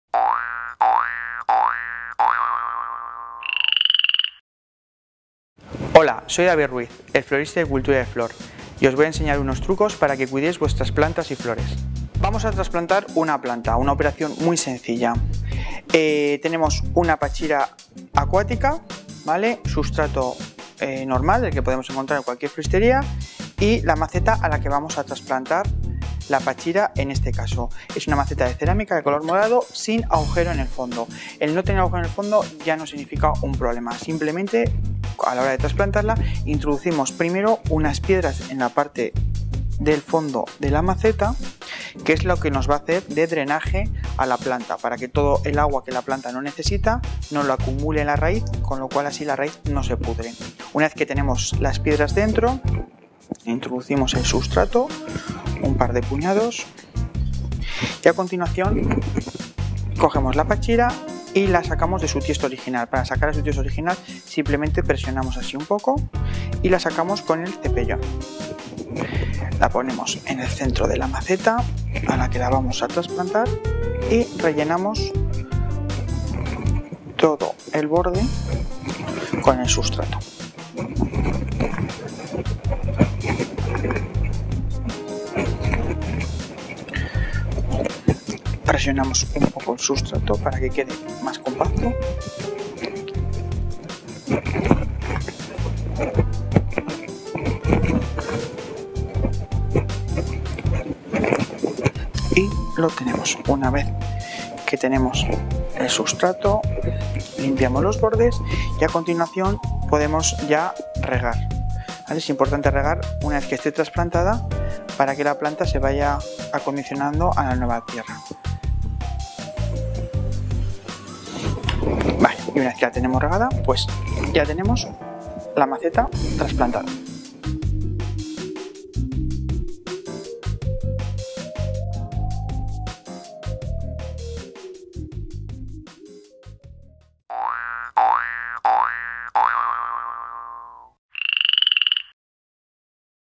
Cómo transplantar una planta
David, de Cultura de Flor, nos enseña cómo se transplanta una planta, de una manera fácil y clara.

el mayor portal de videoaprendizaje de alta calidad.